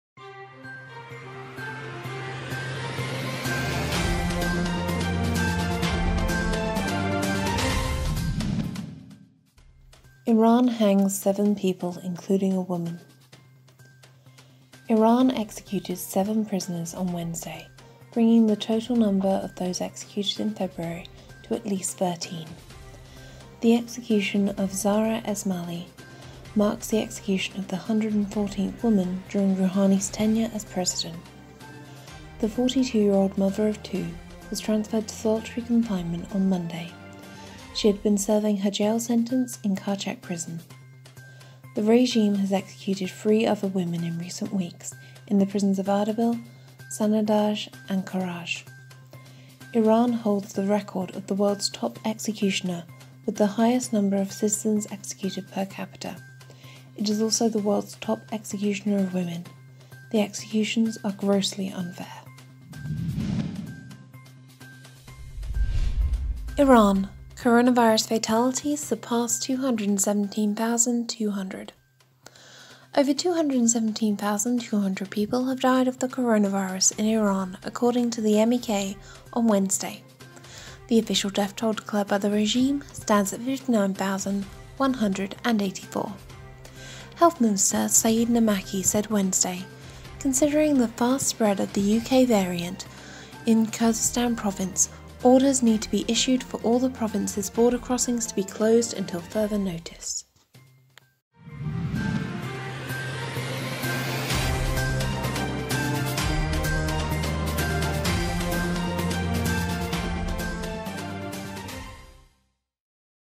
Iran news in brief, February 18, 2021
Iran's Regime Hangs Seven People Including a Woman

Iranian regime executed seven prisoners on Tuesday, bringing the total number of those executions in February to at least 13. The execution of another woman, Zahra Esmaili, marks the execution of the 114th woman during Rouhani’s tenure as the regime’s president.
The 42-year-old mother of two was transferred to solitary confinement in a group of 10 death-row prisoners on Monday, February 15. She had been serving her jail sentence in the notorious Qarchak Prison.
The clerical regime has executed three women in recent weeks in the prisons of Ardabil, Sanandaj and Karaj.
Iran holds the record of the world’s top executioner with the highest number of citizens executed per capita. It is also the world’s top executioner of women. The executions are grossly unfair.

Iran: Coronavirus Fatalities Has Surpassed 217,200

Over 217,200 people have died of the novel coronavirus in 480 cities checkered across all of Iran’s 31 provinces, according to the People's Mojahedin Organization of Iran as of Wednesday. The official death toll declared by the regime stands at 59,184 around a third of the actual figure.
“Considering the fast spread of the mutated UK strain of coronavirus in Khuzestan province, orders need to be issued for all of the province’s border crossings be closed until further notice,” said Health Minister Saied Namaki on Wednesday, according to the regime's official IRNA news agency.

t.ly/MybJ